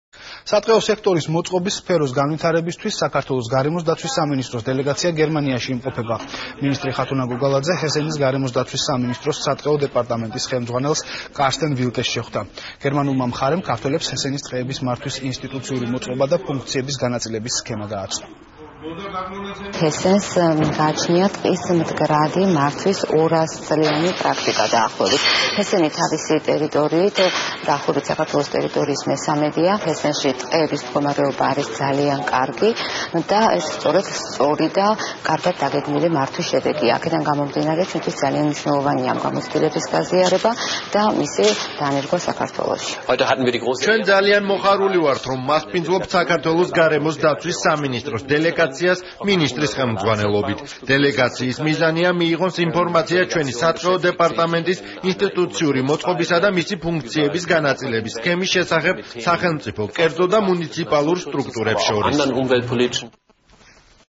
ვიზიტი გერმანიაში
სატყეო სექტორის მოწყობის სფეროს განვითარებისთვის საქართველოს გარემოს დაცვის სამინისტროს დელეგაცია გემრანიაში იმყოფება. მინისტრი, ხათუნა გოგალაძე ჰესენის გარემოს დაცვის სამინისტროს სატყეო დეპარტამენტის ხელმძღვანელს შეხვდა. გერმანულმა მხარემ ქართველებს ჰესენის ტყეების მართვის ინსტიტუციური მოწყობა და ფუნქციების განაწილების სქემა გააცნო.